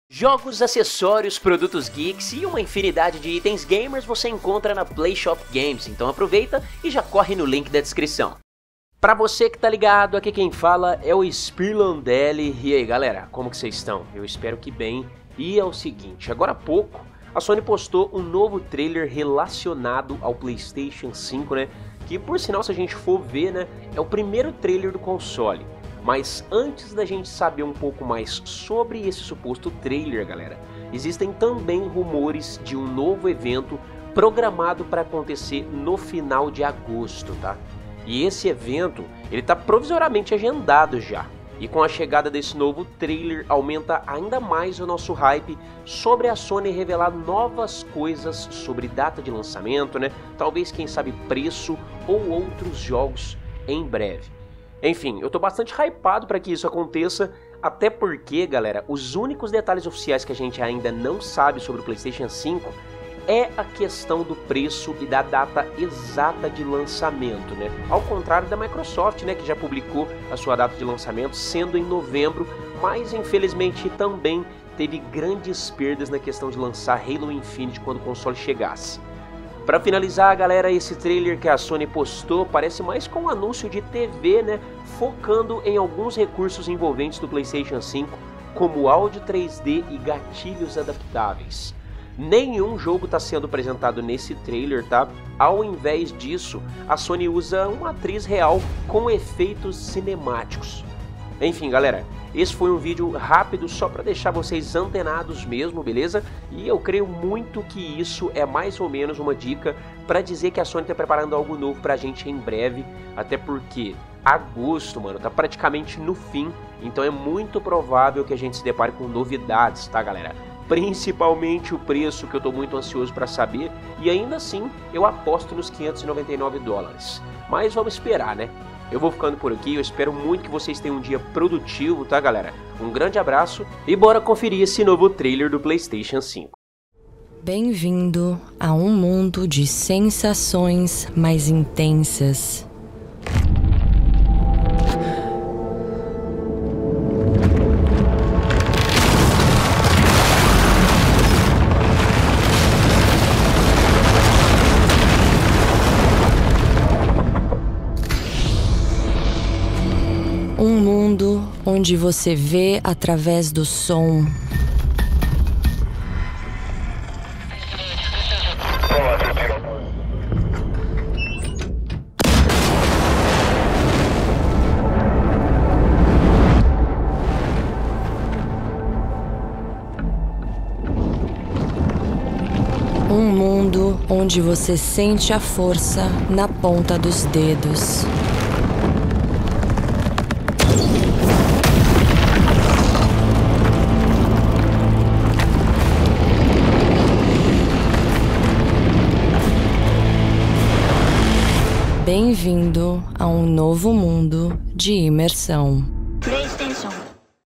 PS5 : NOVO TRAILER É DIVULGADO E EVENTOS PODEM ACONTECER AINDA EM AGOSTO !
Sony divulga novo vídeo sobre algumas especialidades do seu próximo console .